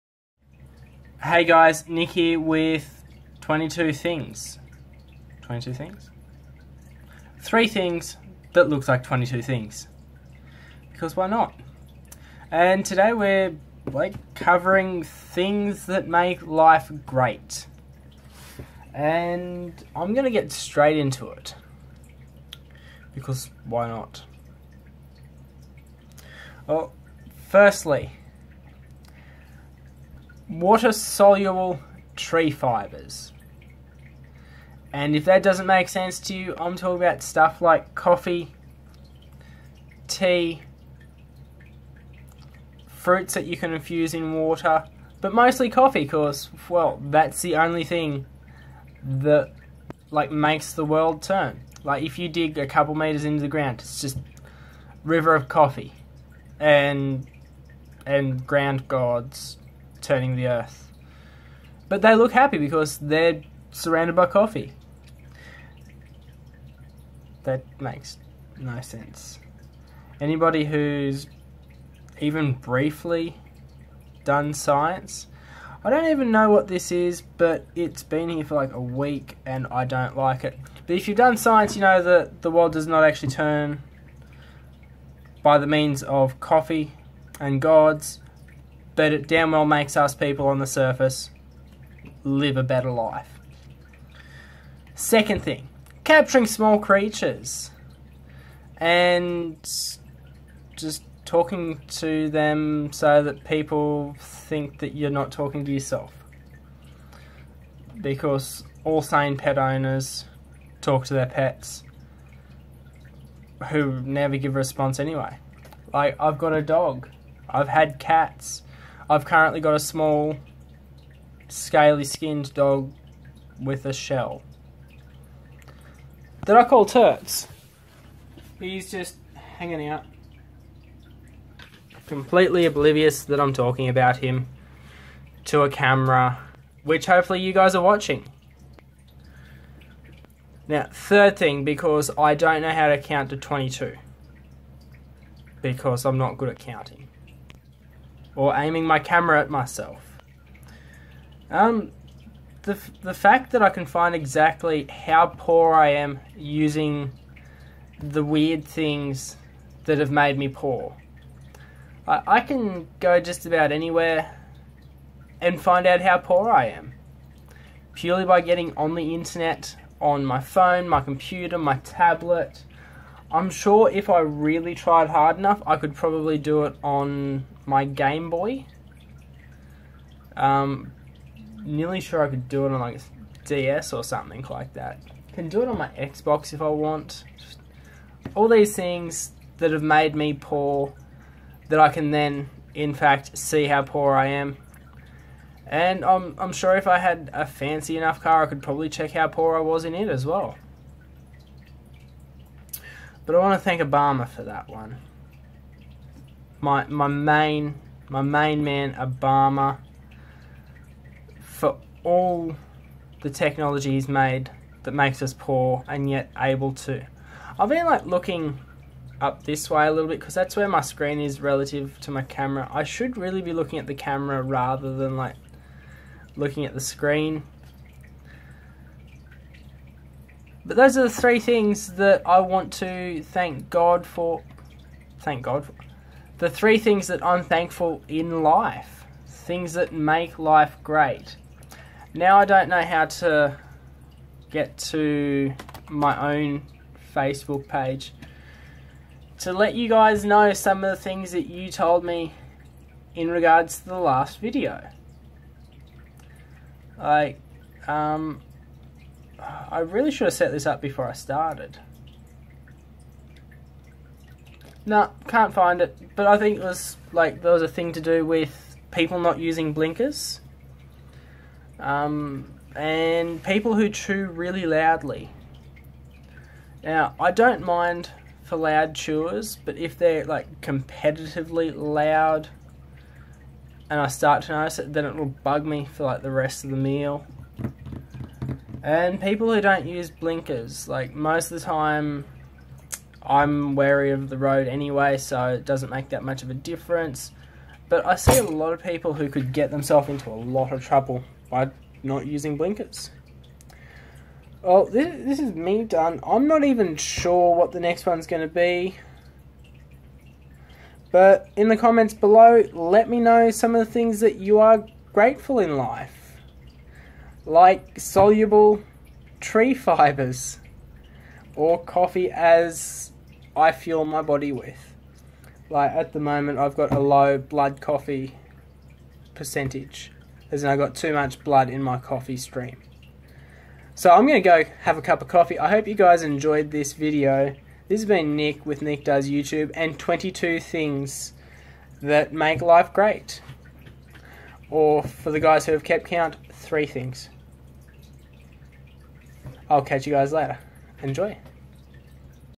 22 Things That Make Life Great | Nic Does Youtube
As the second installment of 22 things, or something like that, we today cover things that make life great.
To keep up with all the fun things going on, head over to my